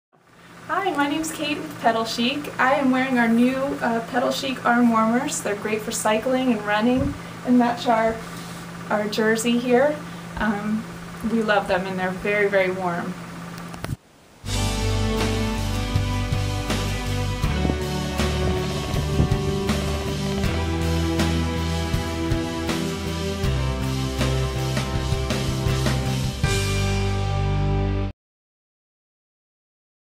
New Pedal Chic Arm Warmers | Great for Biking or Running | Women's Bike Fashion
Get your super cool, super warm, Pedal Chic arm warmers They match our short sleeve jersey! Perfect for the winter months for cycling and running.  Easy on and off to maintain optimal comfort on your run or ride while looking very stylish.  Made by Primal.  Check out more great bike fashion in our new online boutique.